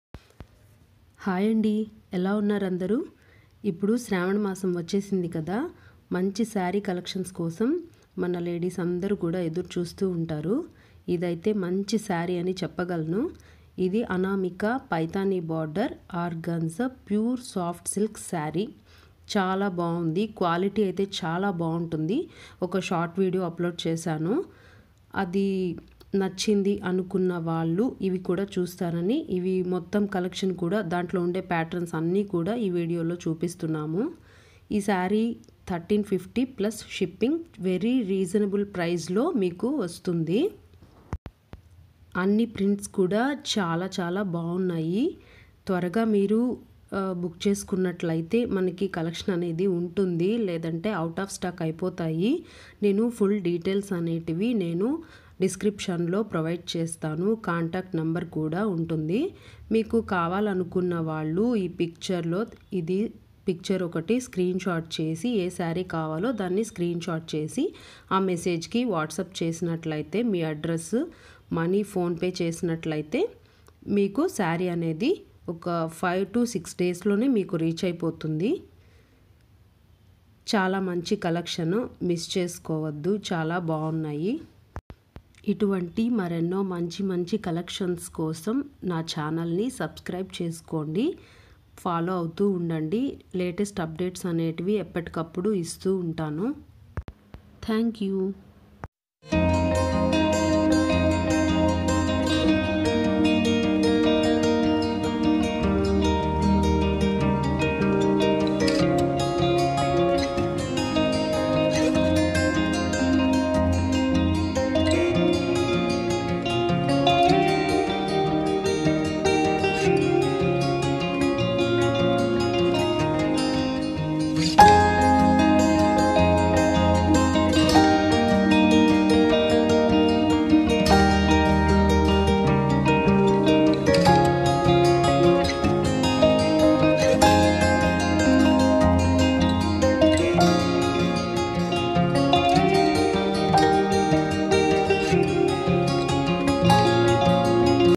gold color pythani border organza saree.1350+s.
Anamika Paithani Border Orgenza Soft Silk Party Wear Saree

We take pride and are excited to introduce these royal ethnic sarees to you!

It's highly handpicked and designed exclusively just for you!
Exult this festival/wedding season with these incredible weaved of pure Organza silk sarees, that is high in quality and weaved all over the body with Gold Zari work And majestic contrast very big border and royal ombre kinda pallu.
It pairs with a contrasting blouse with a border to make them look even more classic and graceful.
Saree Cut: 5.50 mt
Saree Blouse: 0.80 mt
|| Easy Hand Wash ||
|| 100% Quality assurance Item ||